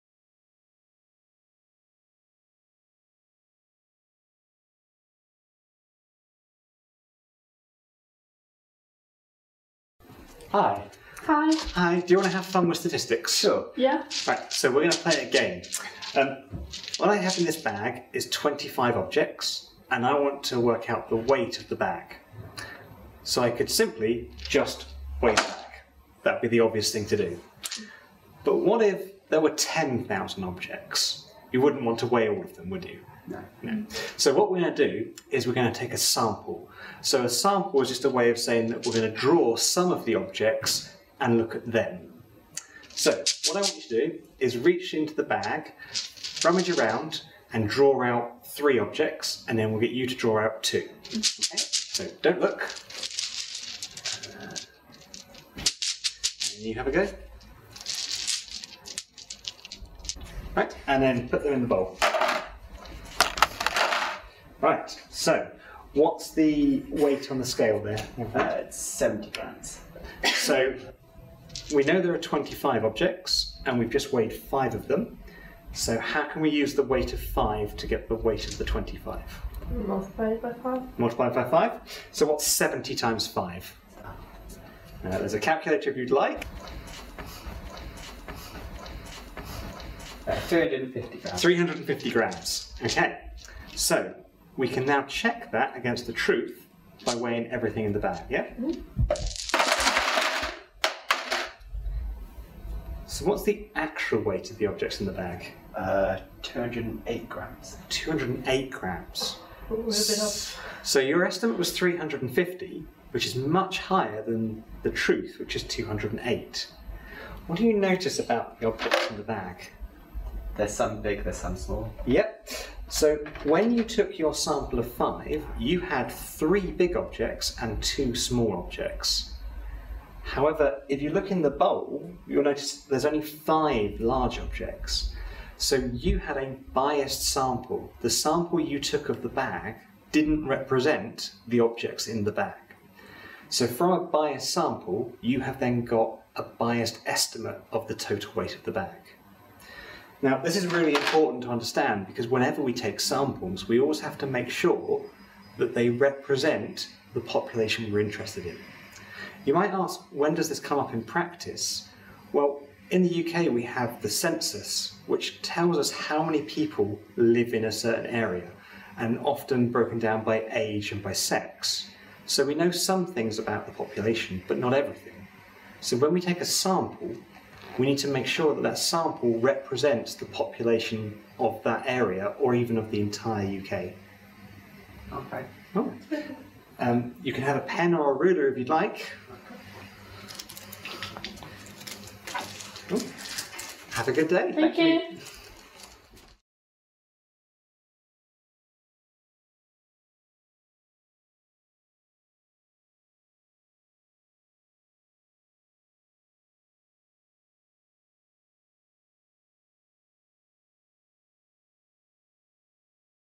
Hands-on statistics – Biased sampling
Hands-on statistics activities have been developed for our members to use in careers fairs, at festivals and in schools. This video demonstrates one of the activities in use. Written instructions for this hands-on activity are available